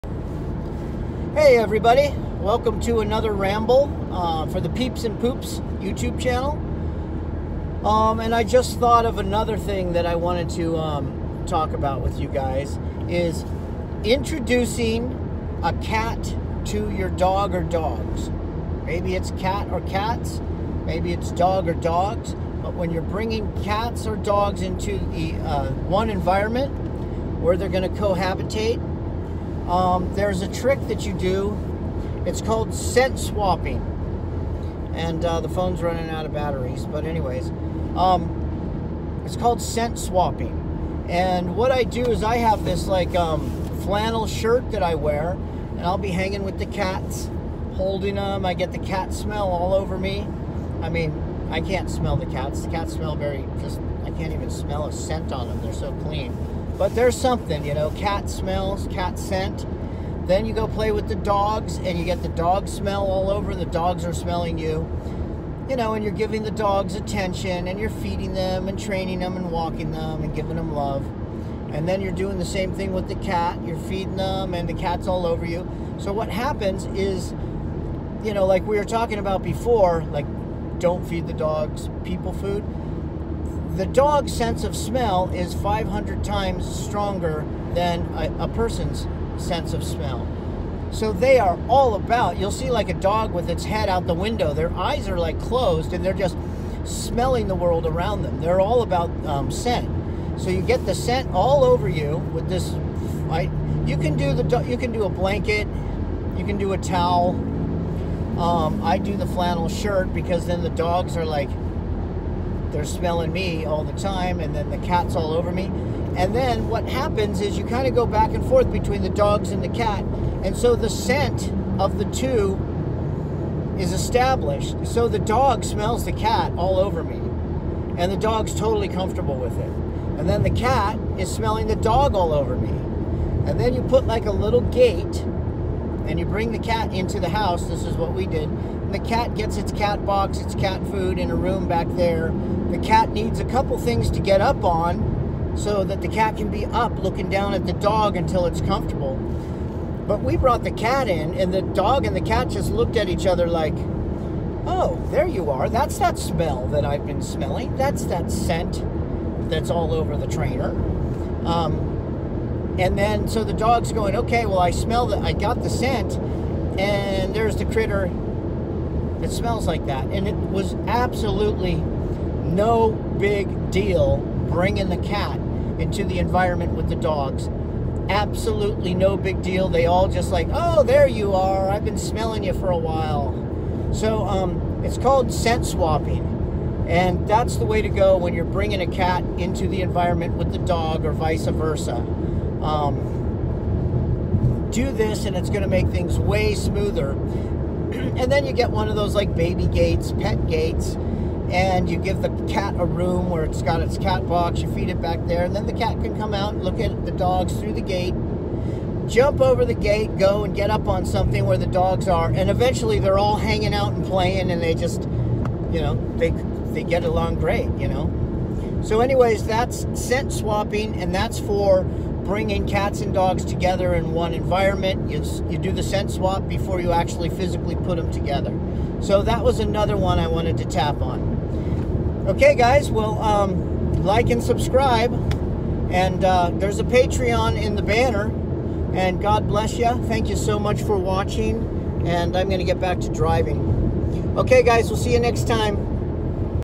How to Introduce Cats & Dogs
How to Introduce Cats & Dogs to eachother other with Scent Swapping